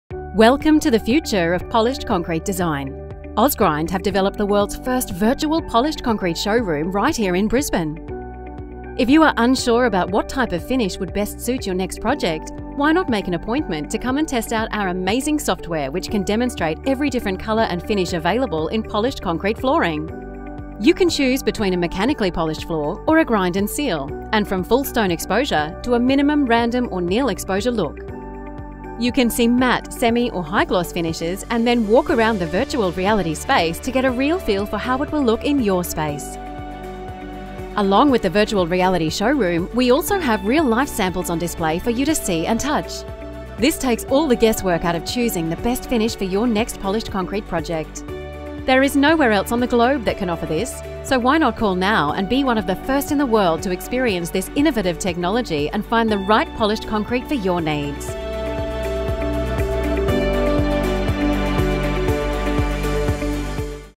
OzGrind Polished Concrete Virtual Reality Showroom Brisbane Ad
World first virtual polished concrete showroom by OzGrind.  Take a tour of this innovative technology used to demonstrate every type of polished concrete flooring available.